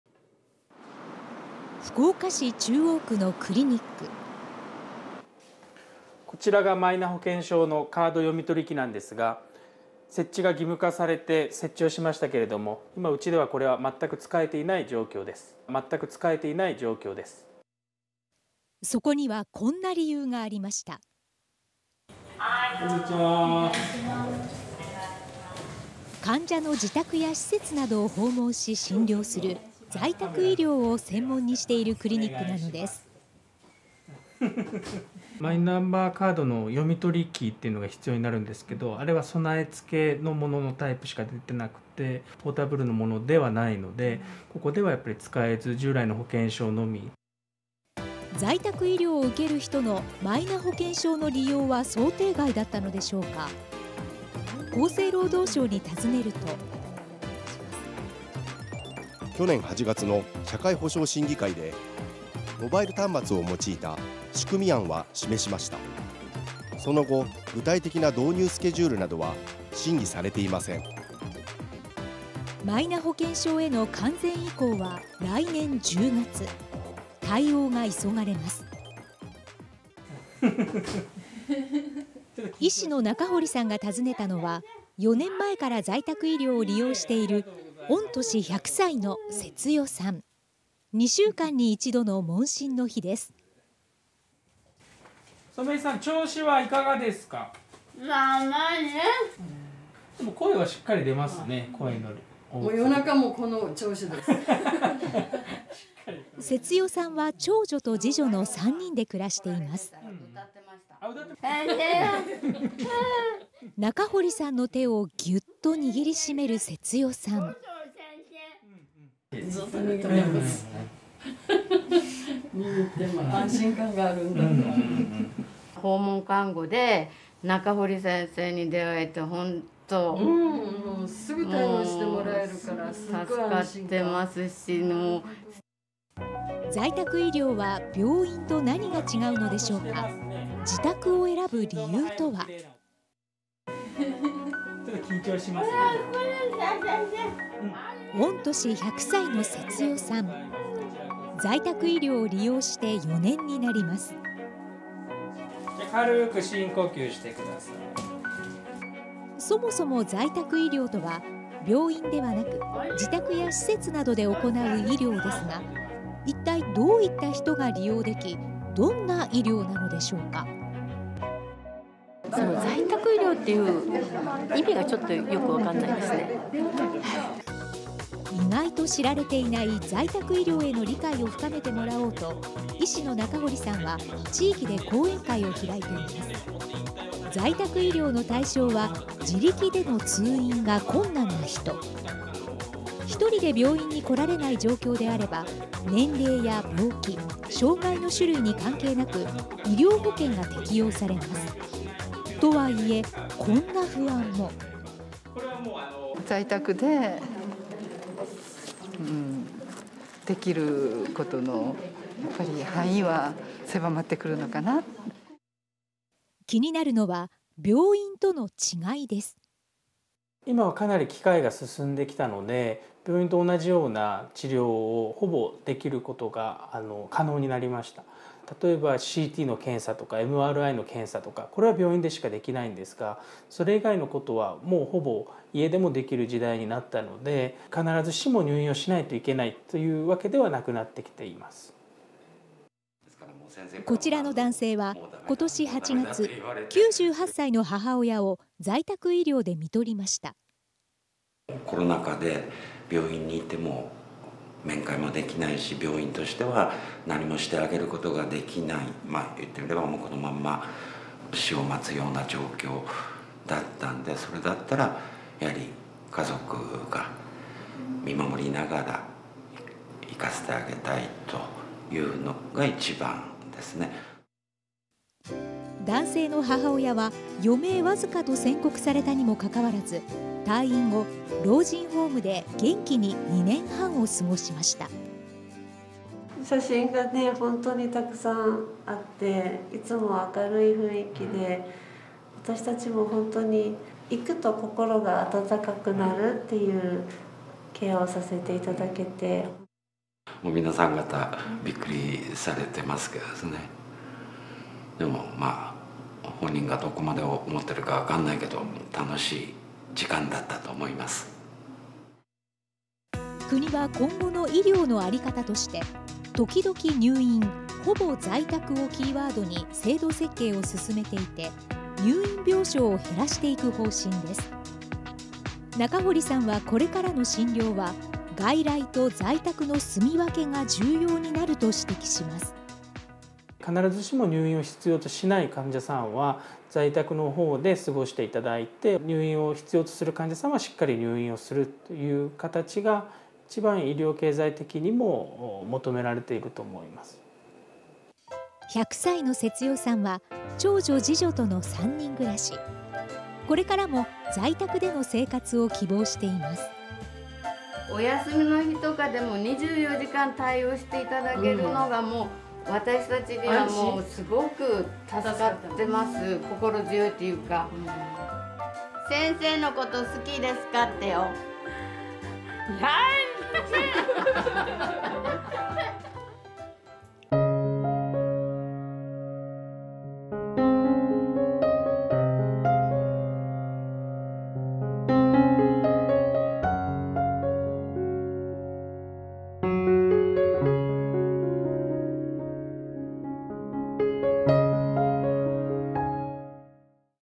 「ときどき入院 ほぼ在宅」増える在宅医療 マイナ保険証が使えない！？
2024年秋にはマイナンバー健康保険証に完全移行する予定ですが、訪問診療の際にマイナ保険証が利用できない状況が発生しています。持ち運べる読み取り機がないためです。在宅医療は病院に近い治療が可能になってきていて、国は入院病床を減らす方針で、在宅医療の重要性は増々高まっています。
2023年10月4日